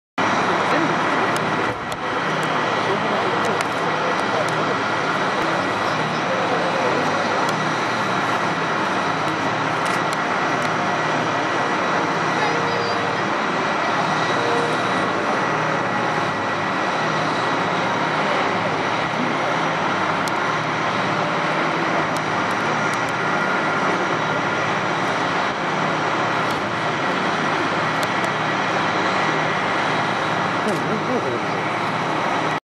ブルーインパルス 入間基地航空祭2012 出発前
２０１２年１１月３日撮影。
入間基地航空祭。ブルーインパルス。飛行出発前。